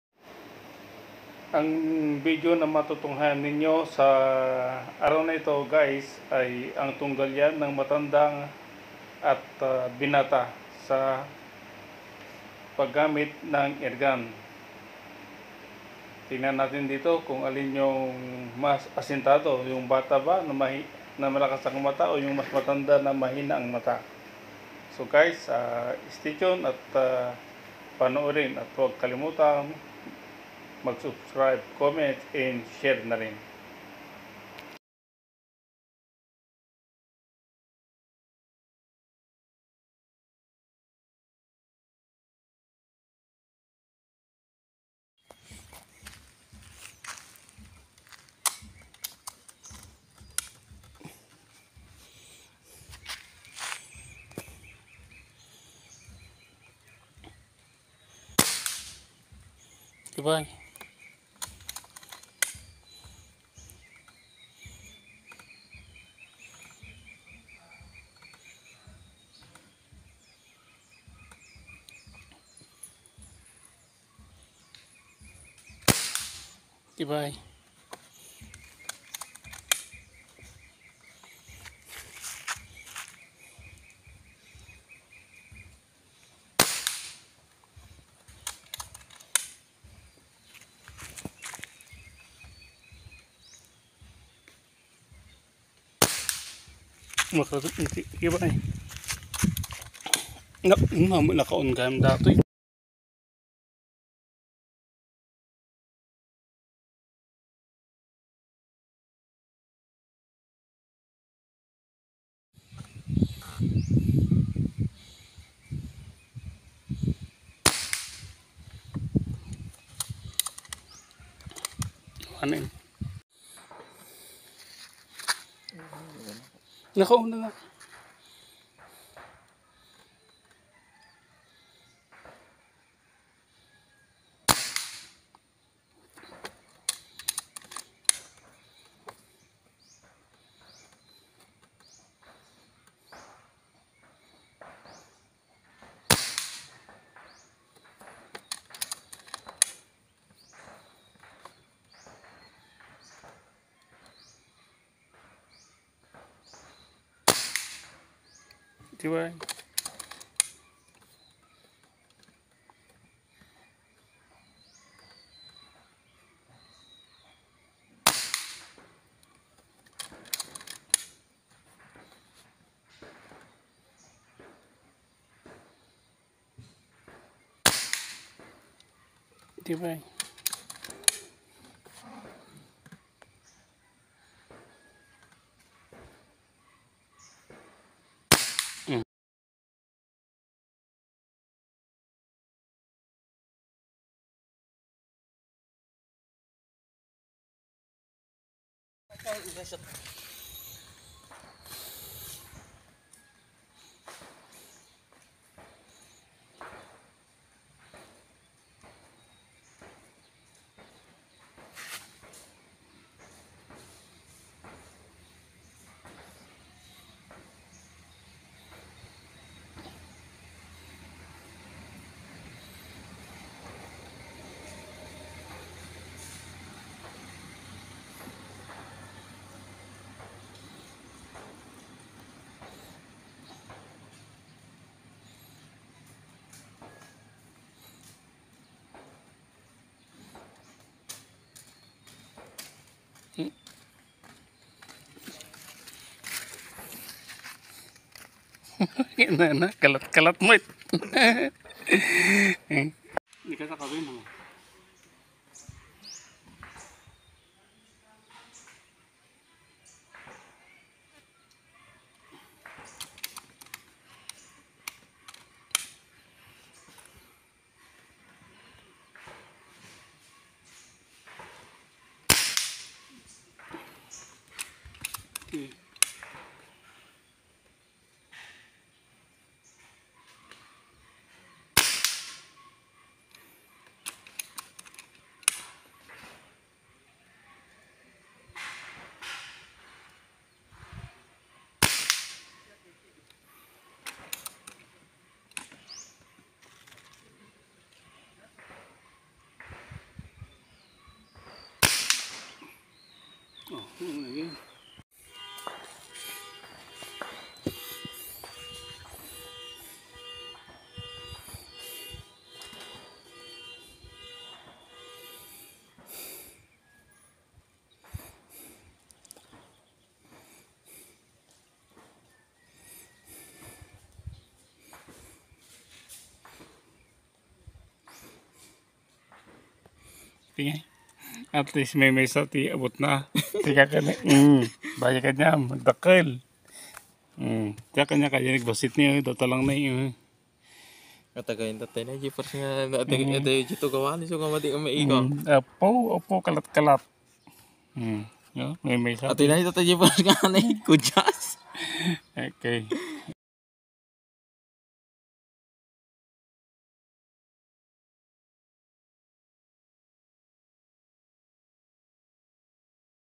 Pagalingan sa pagTarget (Binata versus Matanda)
Pahusyan kung sino ang malabo at malakas ang pulso. Yung binata o yung matanda?